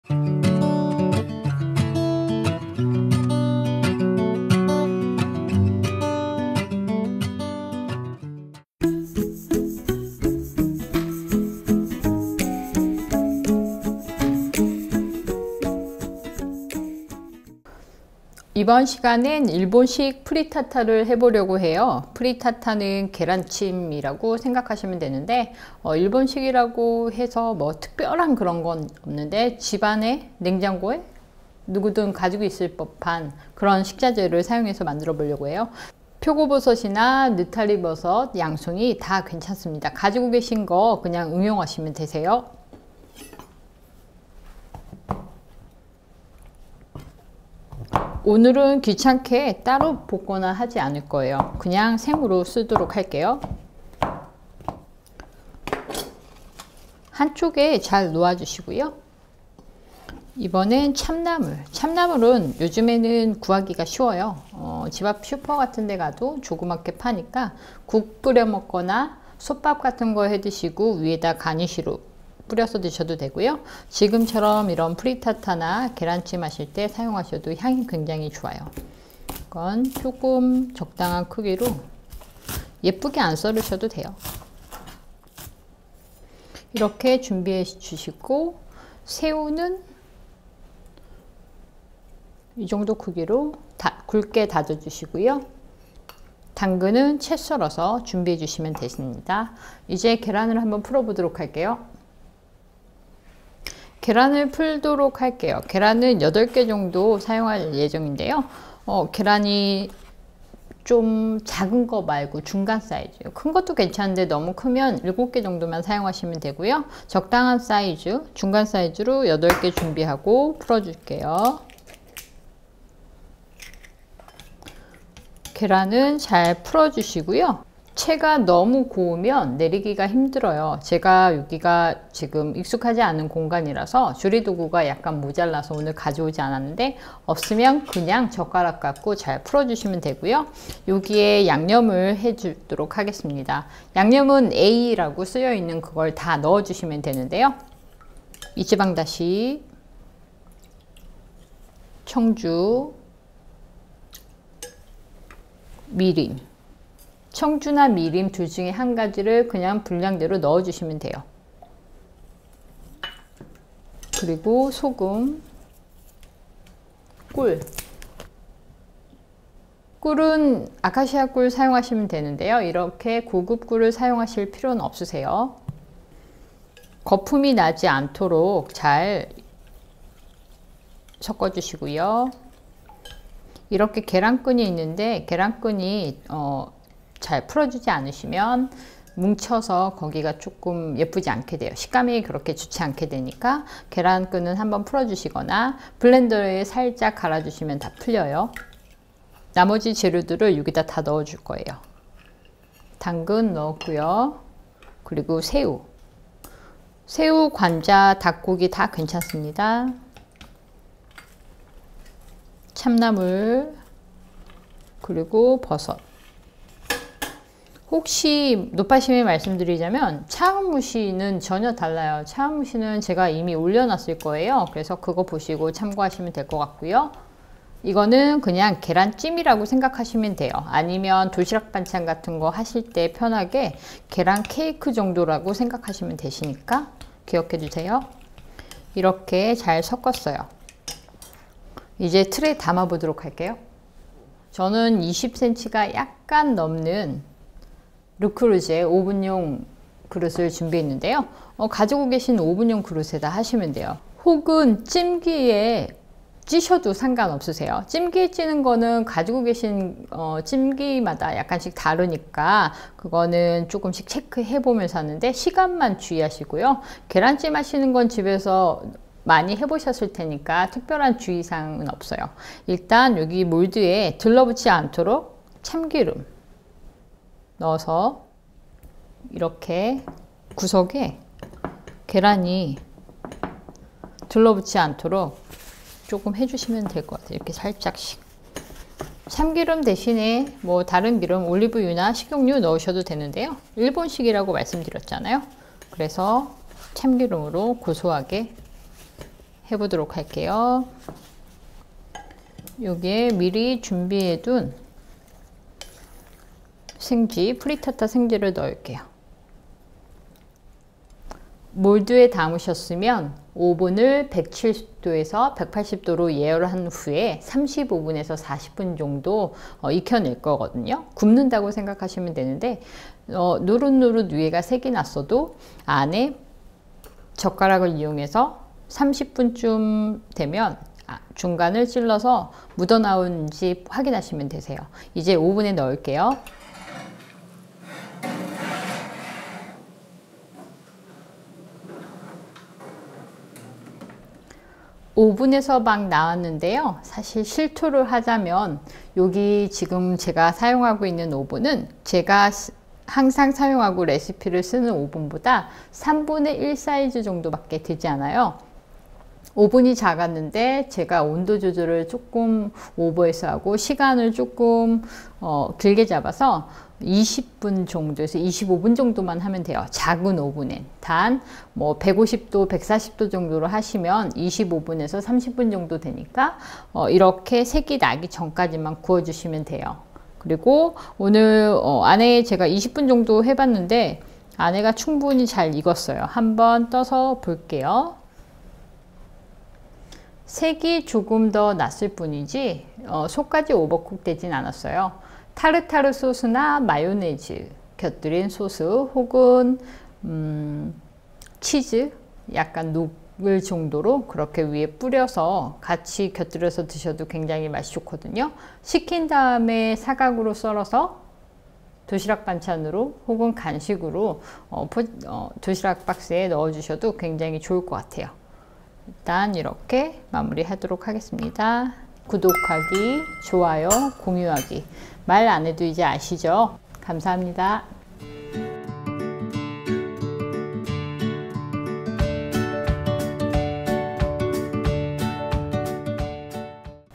일본식 프리타타 - 계란 오믈렛을 맛있게 만드는 방법
[재료]
표고버섯 3~4개, 당근 60g, 참나물 7줄기 굵게 채썬 것, 새우 3~5마리(껍질 깐 것), 참기름1T

A: 달걀 8개, 이찌방다시 50ml, 청주 1T, 미림 1T, 소금 1/5t, 꿀1T

이 영상은 다운로드 및 2차 편집을 금합니다.